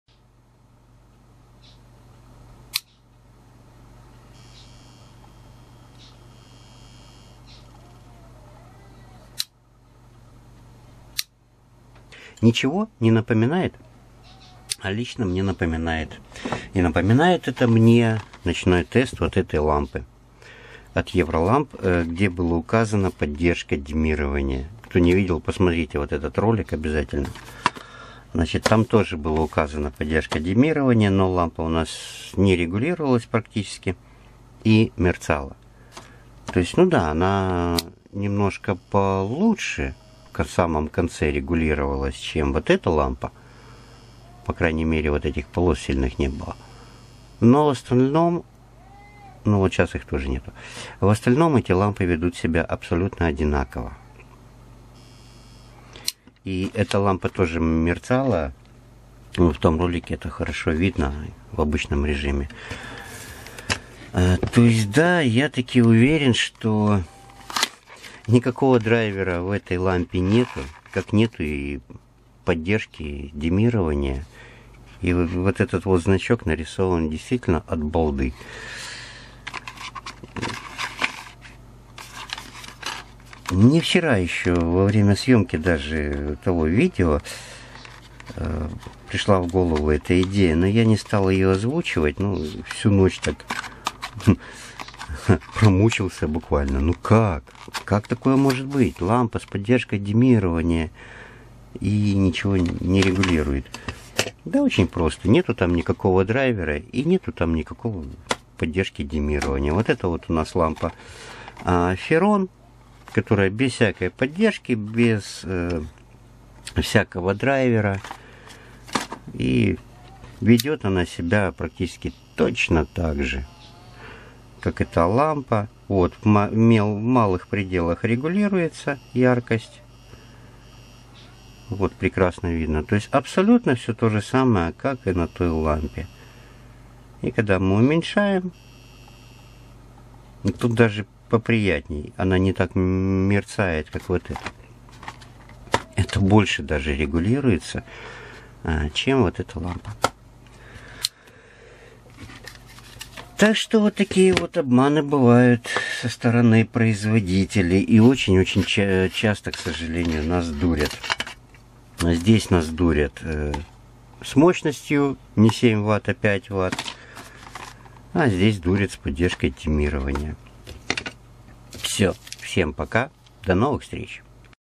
Простая светодиодная лампа и диммер
Как ведёт себя простая светодиодная лампа подключенная через диммер.